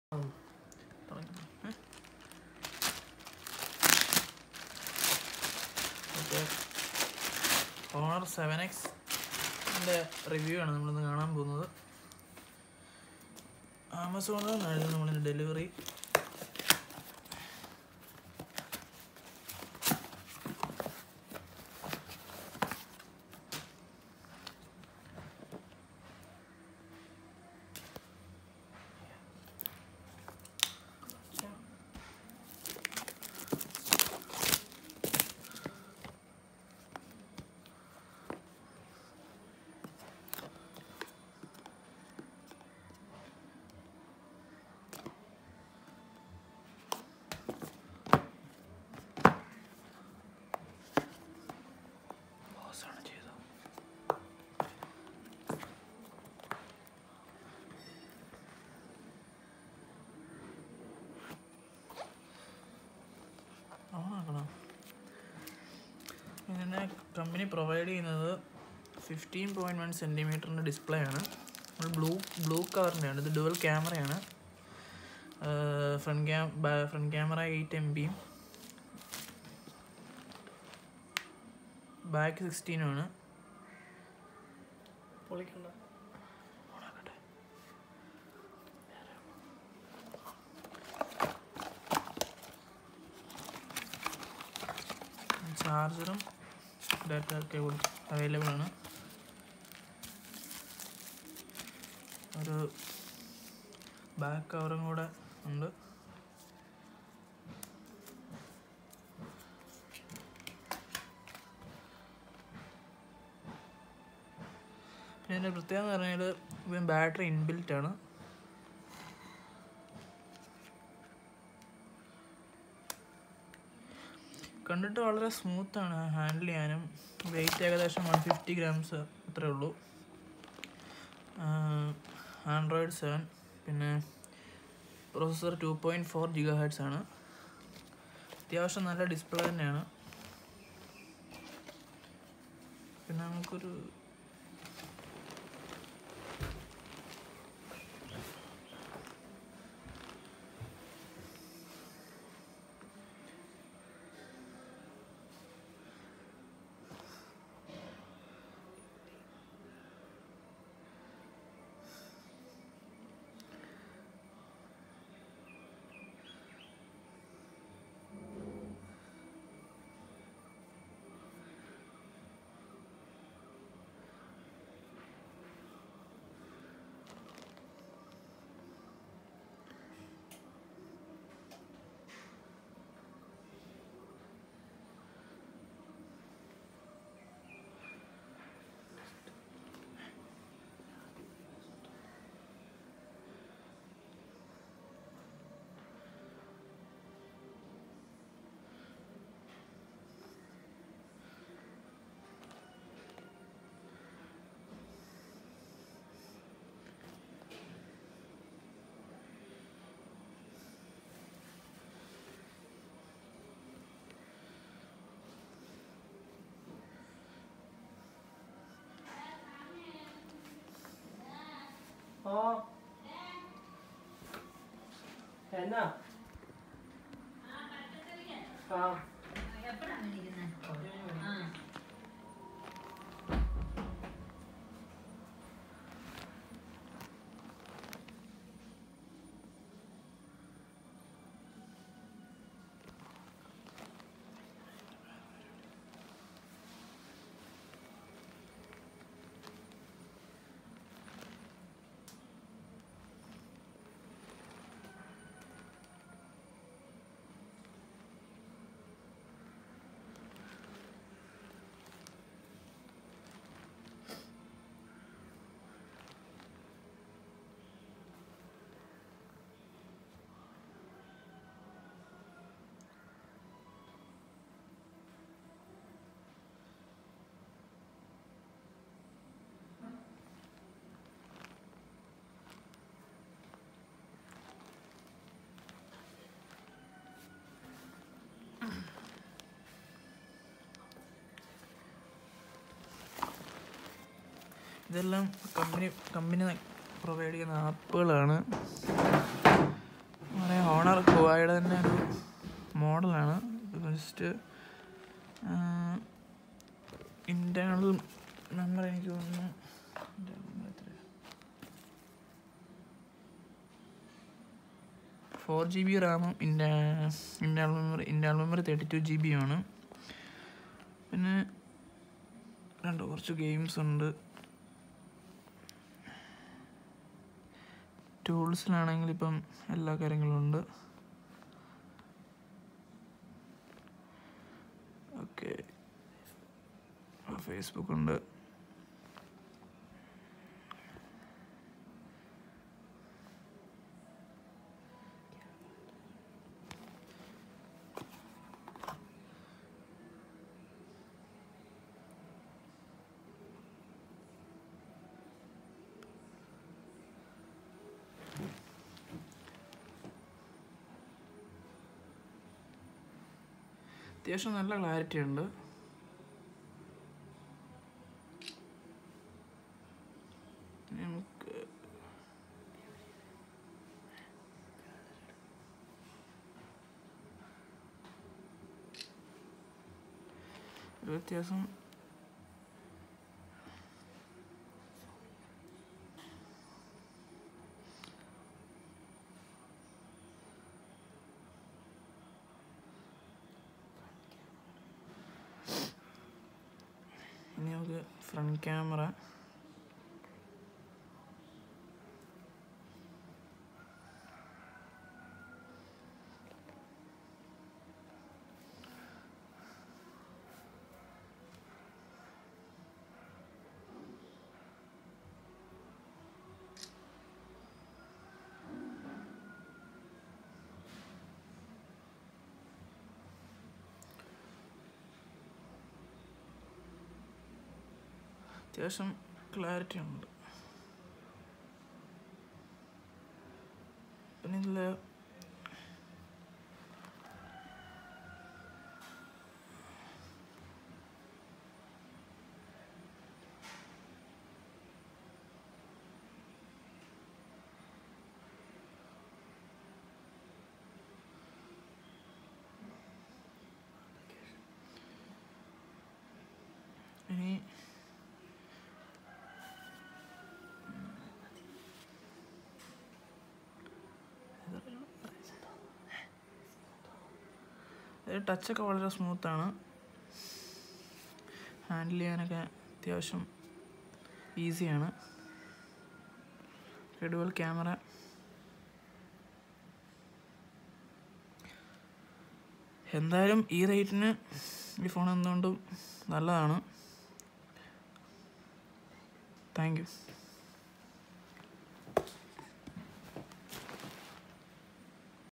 Huawei Honor 7X (Blue, 4GB RAM + 32GB Memory) unboxing and Review
Technical Details
OS: Android
RAM: 4 GB
Item Weight: 163 g